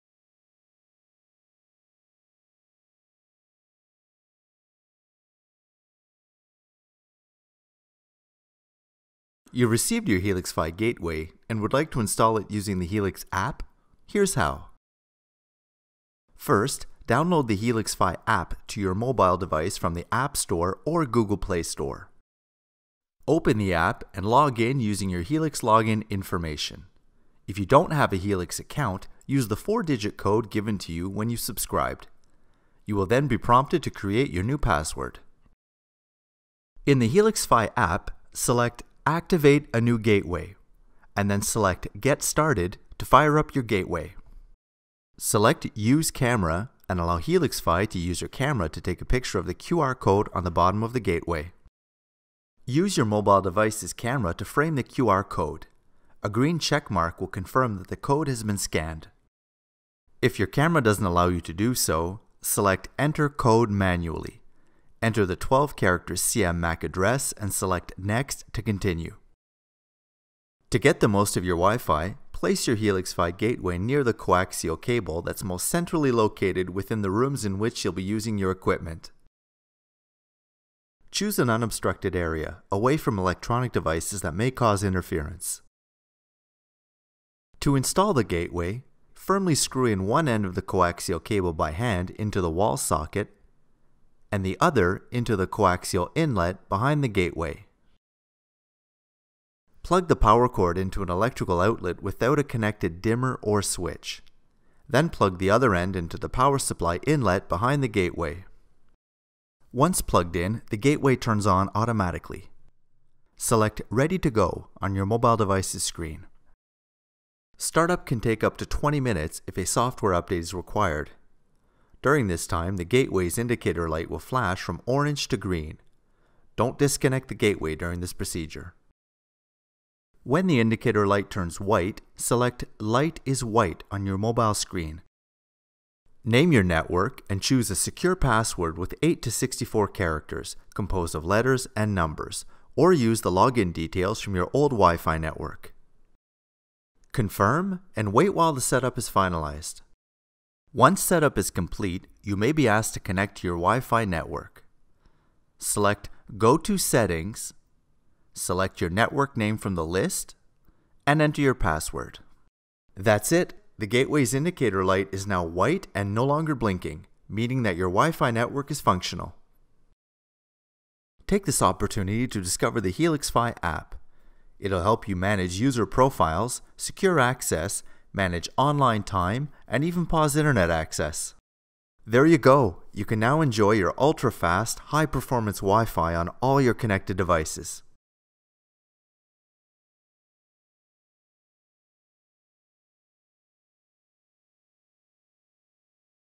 Helix tutorial | How to install the Helix Fi gateway with the Helix Fi app?
Learn how to easily install your Helix Fi gateway with the Helix Fi app.